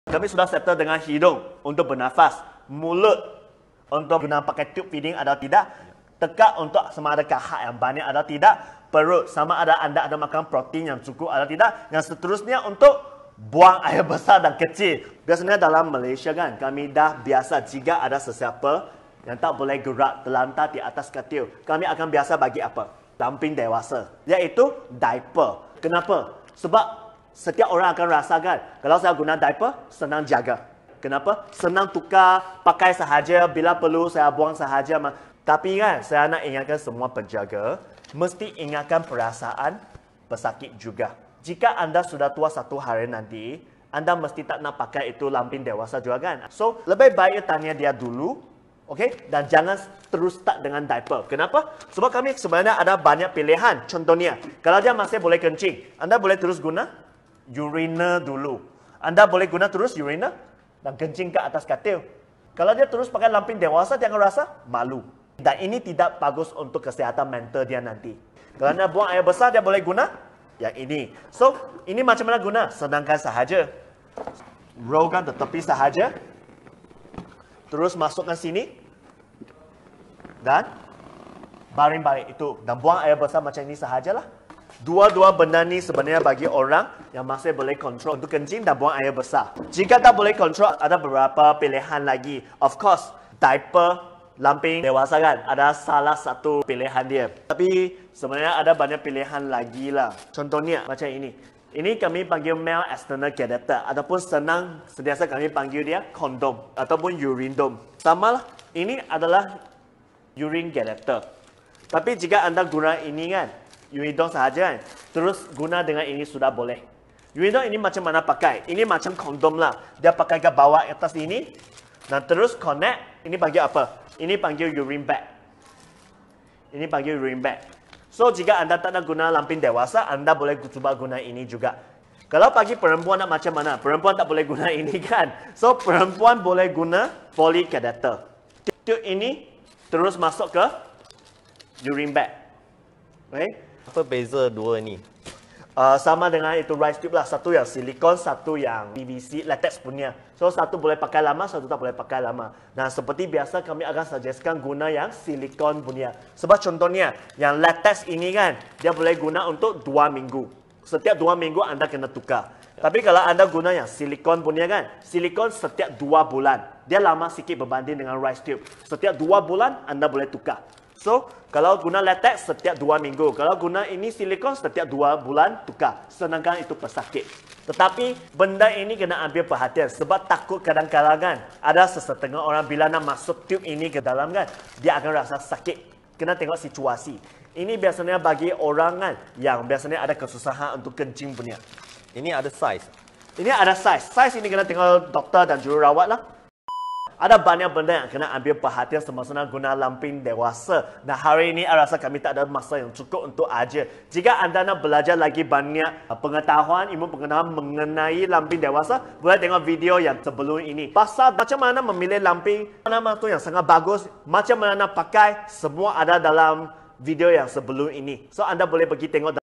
【Discaj dari hospital 】Part 5 | Buang air besar dan kecil
Adakah pesakit harus menggunakan lampin dewasa?
Sekiranya pesakit dapat buang air besar dan kecil sendiri, apakah peralatan yang boleh disediakan untuk mereka?
Bilakah pesakit perlu menggunakan lampin dewasa?
Perkara yang perlu diberikan perhatian semasa menggunakan lampin dewasa ⚠️

Biar Pakar Rehabilitasi Menerangkan Untuk Anda!
Part 5 【Buang air besar dan kecil】

Filial Piety